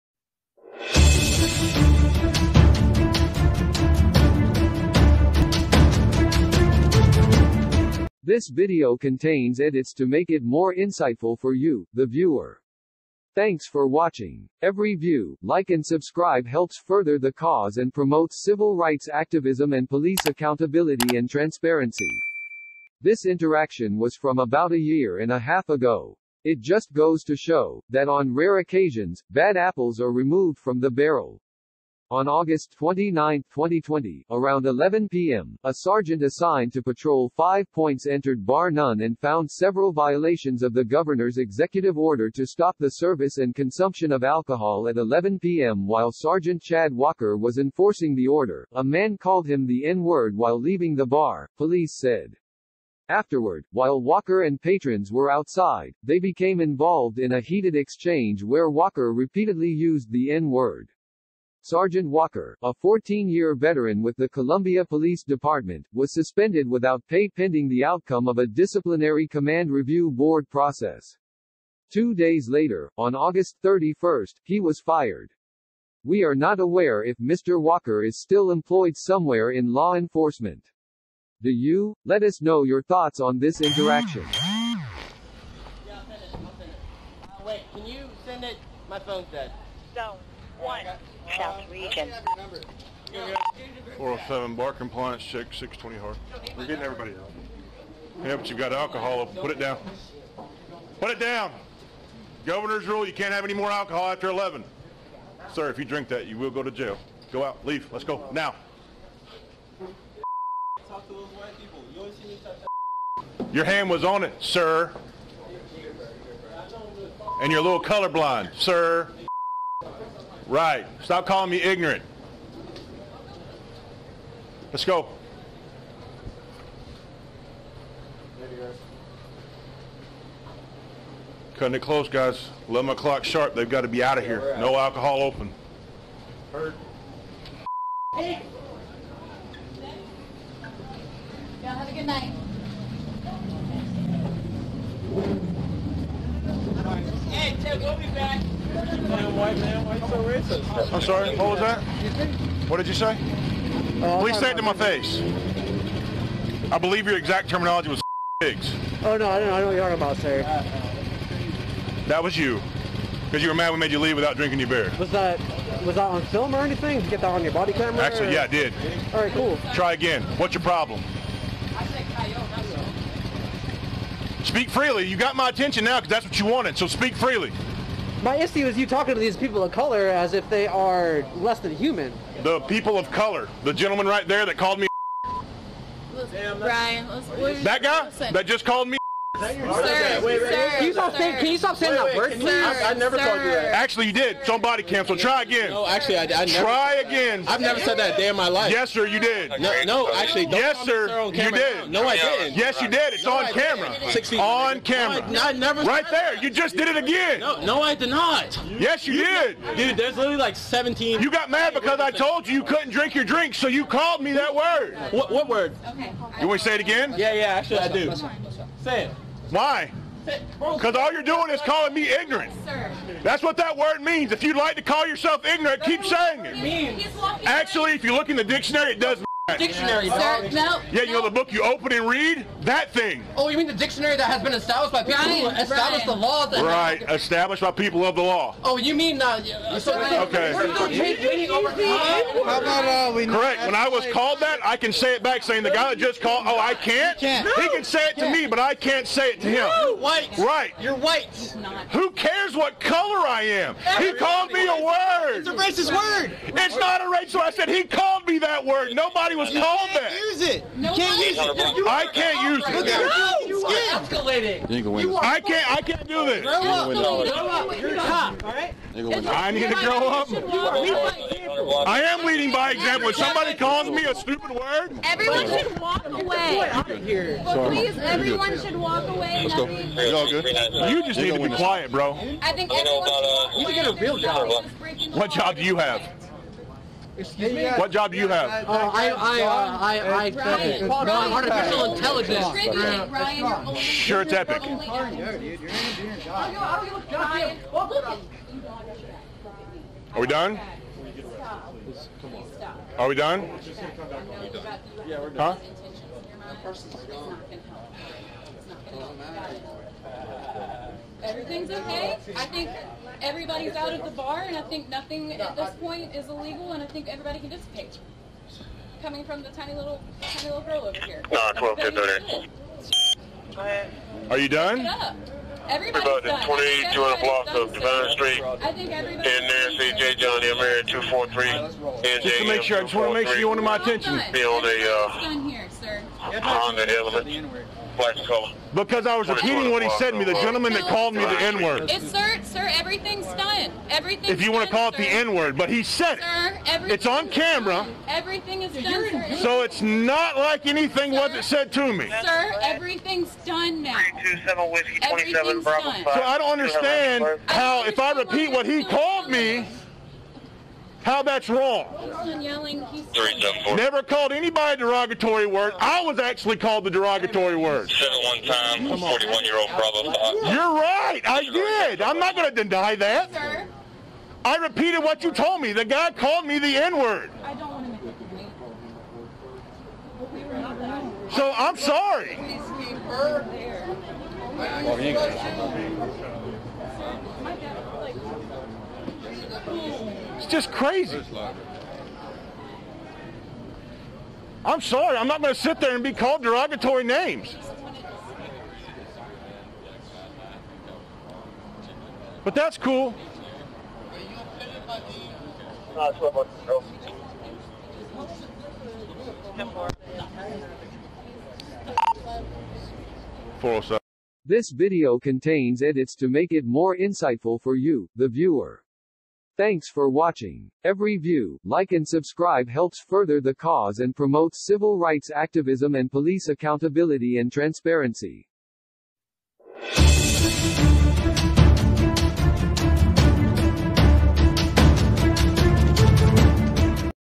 Police Harassment In SC - Tyrant Suspended Then FIRED - Know Your Rights And Always Film The Police
America's Must See News videos